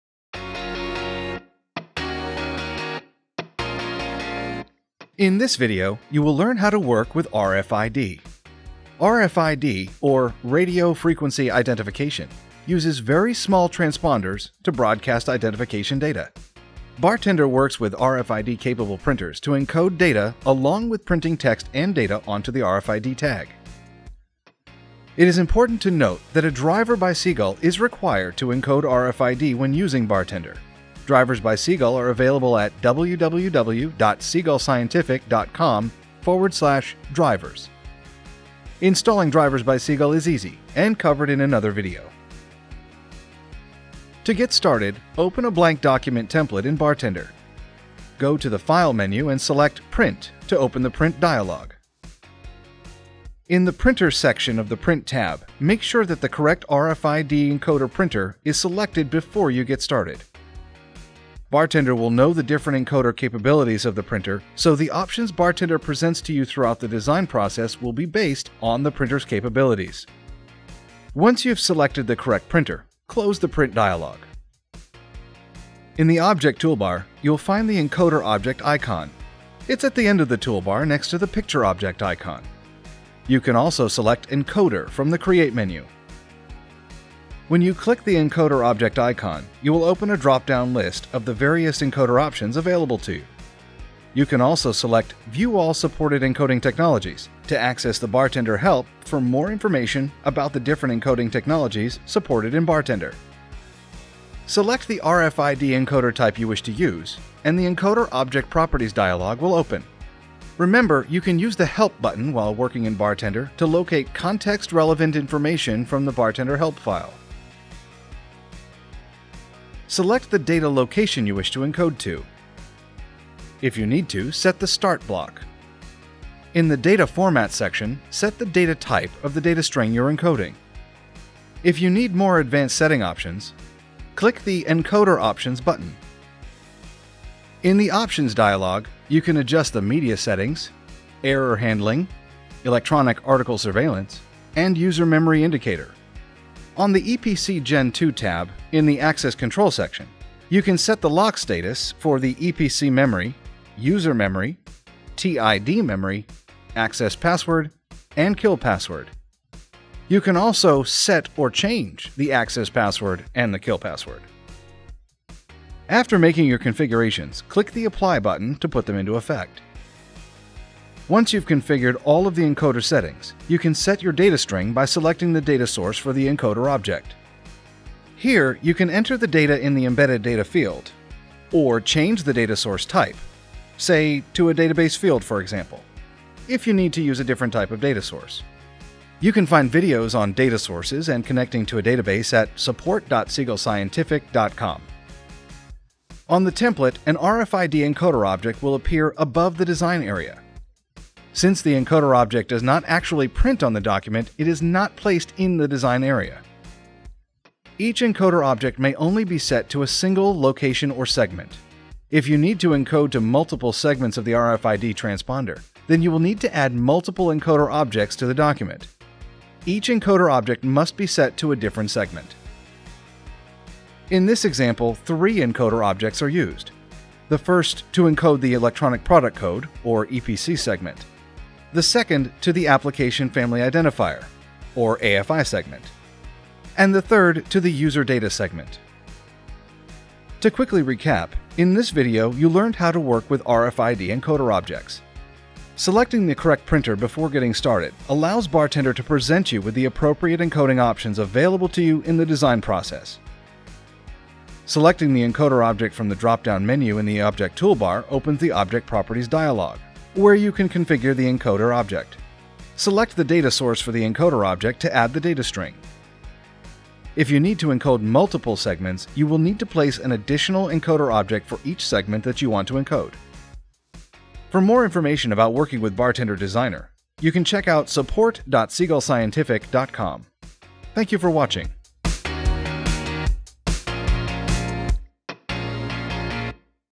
Working with RFID Encoder Objects
Learn how BarTender can encode RFID data onto RFID tags by watching BarTender's video on template design.